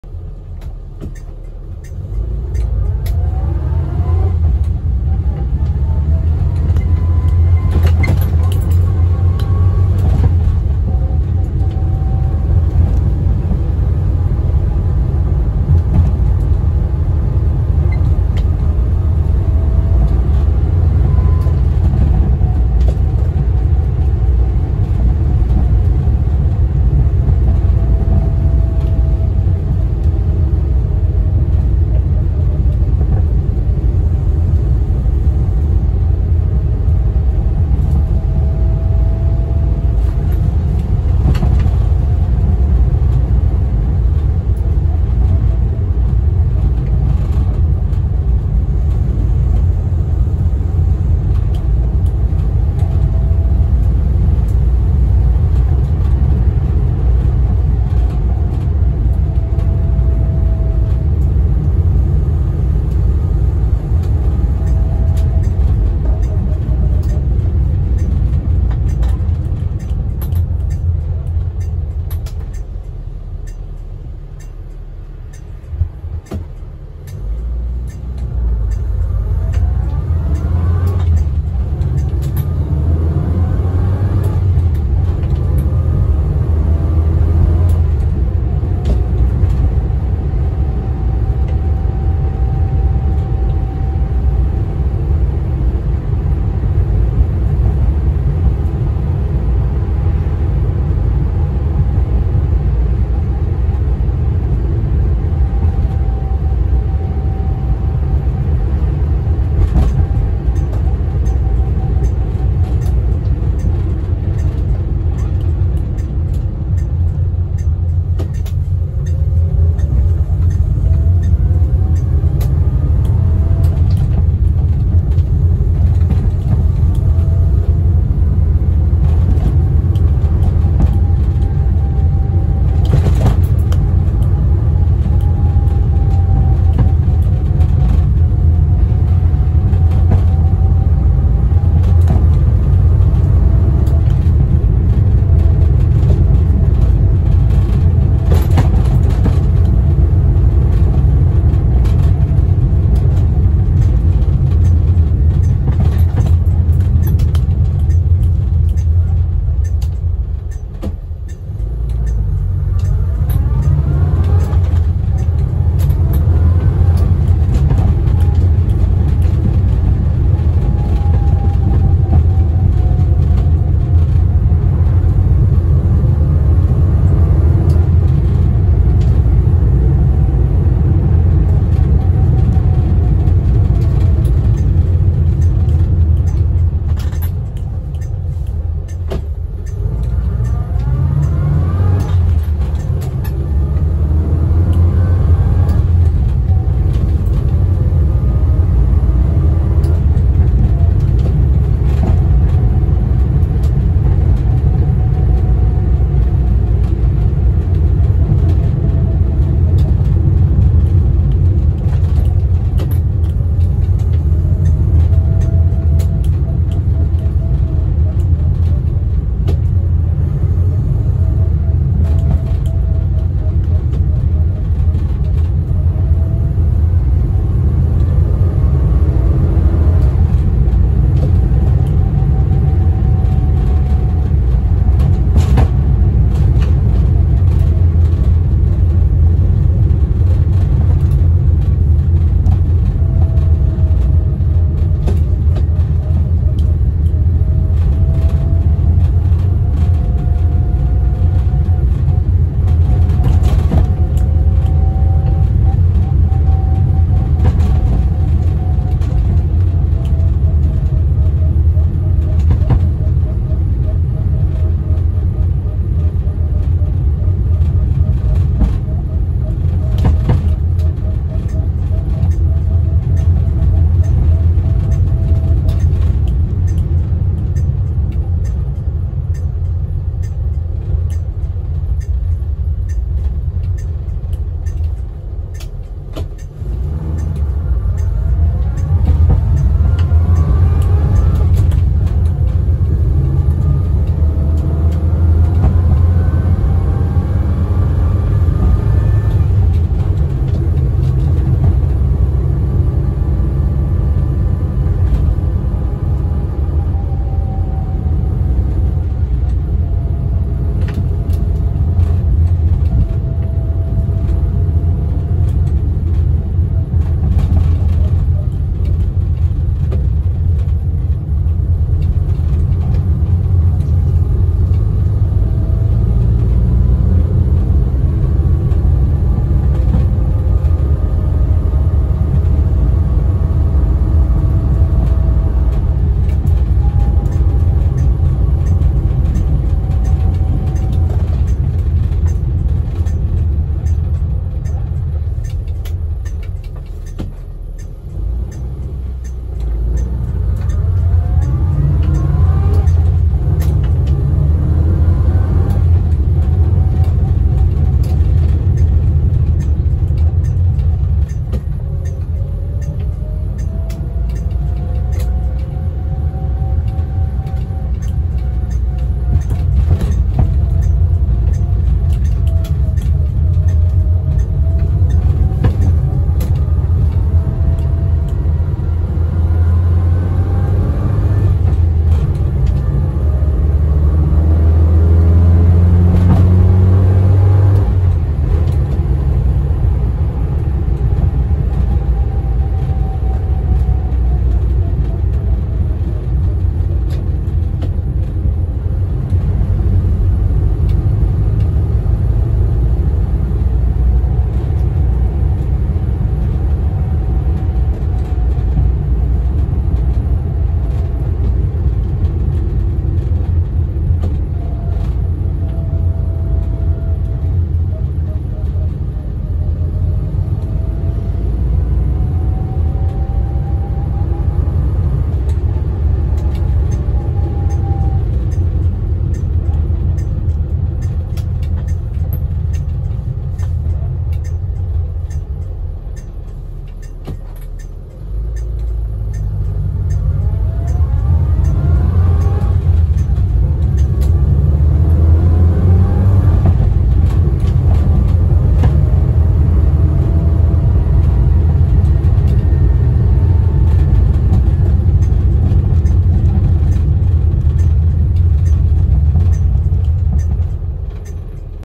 Driving around in a 1975 Austin Allegro
Took my little 1975 MK1 Allegro 1098cc on a little spin around the block! 😁👍🚗🚗🚗

I highly recommend setting the quality to 1080HD for a clearer image and sound!
For those who appreciate and love the Austin/BMC/Morris legendary A series 4 speed gearbox whine, crank up your volume!